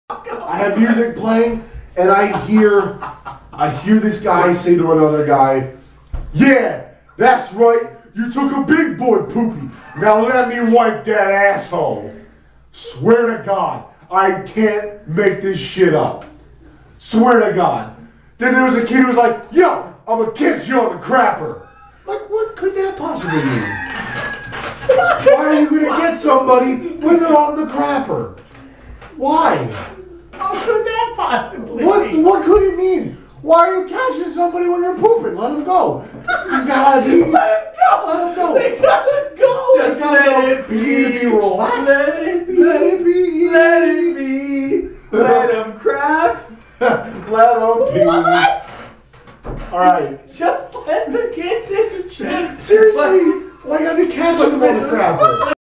Story time with Joe
Joe tells us a story about life in his dorm.